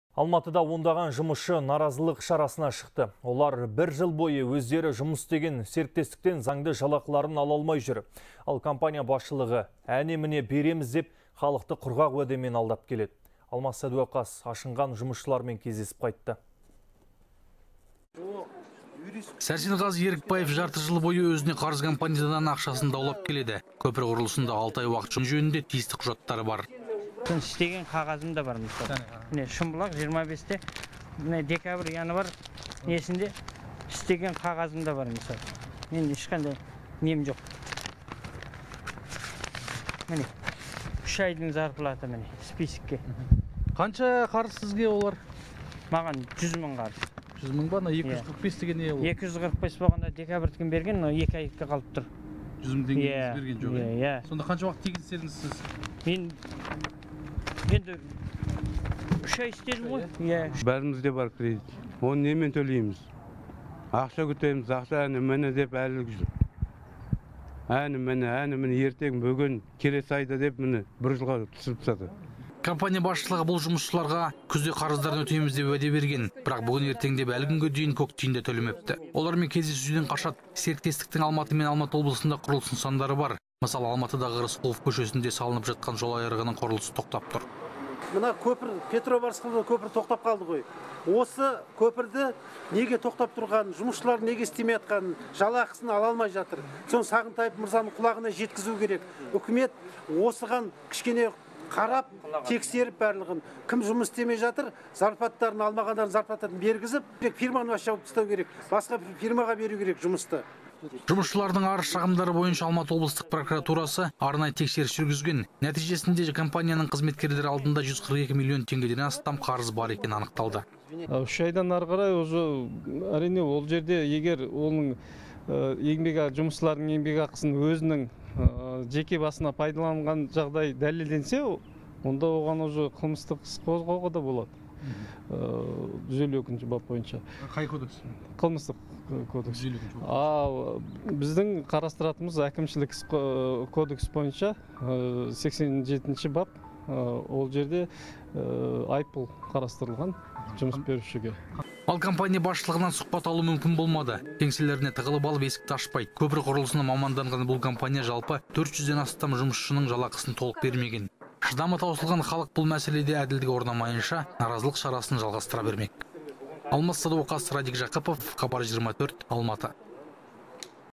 Алматыда көпір салған жұмысшылар 1 жылдан бері жалақысын ала алмай жүр
Алматыда ондаған жұмысшы наразылық шарасына шықты. Олар  бір жыл бойы өздері жұмыс істеген серіктестіктен заңды жалақыларын ала алмай жүр. Ал  компания басшылығы әне-міне береміз деп, халықты уәдемен алдап келеді.

Сәрсенғазы Ерікбаев жарты жыл бойы оған қарыз компаниядан ақшасын даулап келеді. Көпір құрылысында 6 ай уақыт жұмыс істегені жөнінде тиісті құжаттары бар.